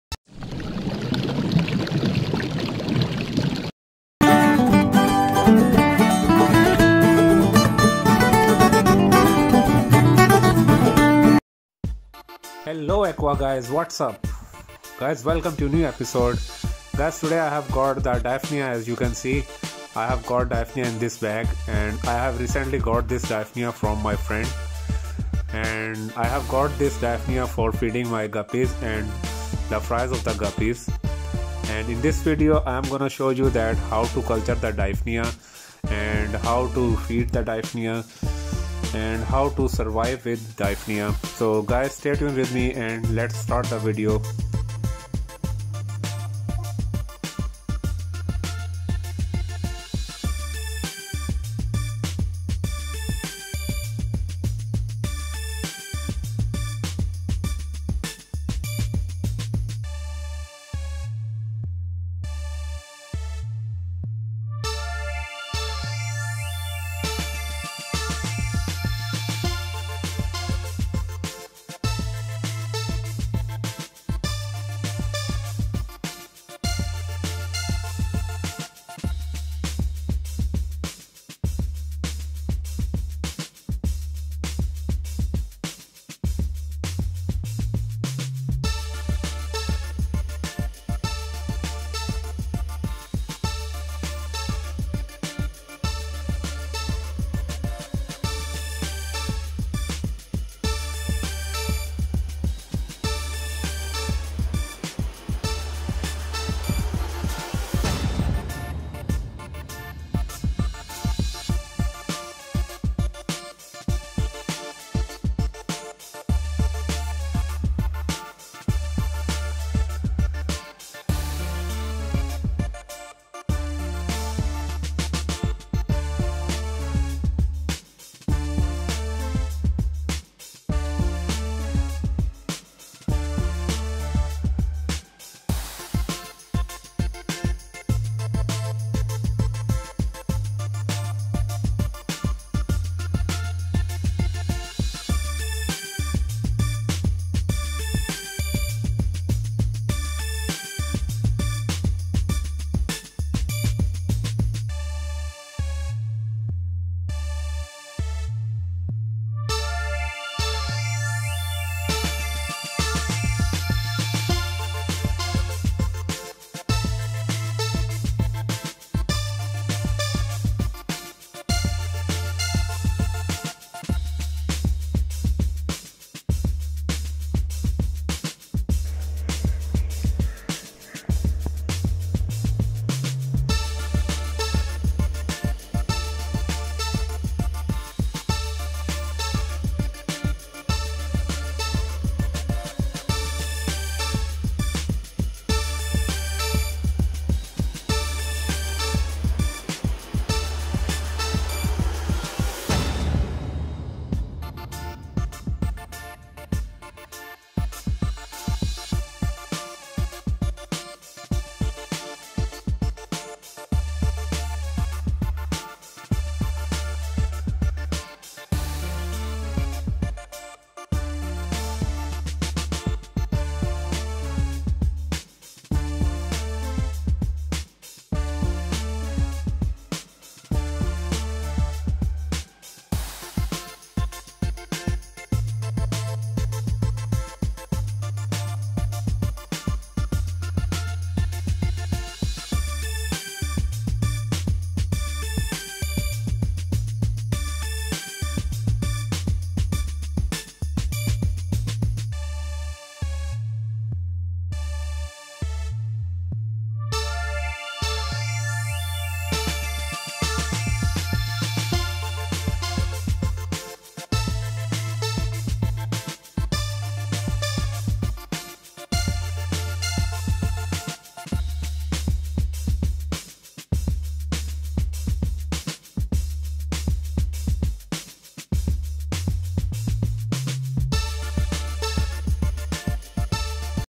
Daphnia Magna|How to Culture Daphnia Easy way?|Culturing Daphnia using Green water
This video is about Daphnia and

Our purpose of making such compilations of pictures and videos is to promote knowledge and information.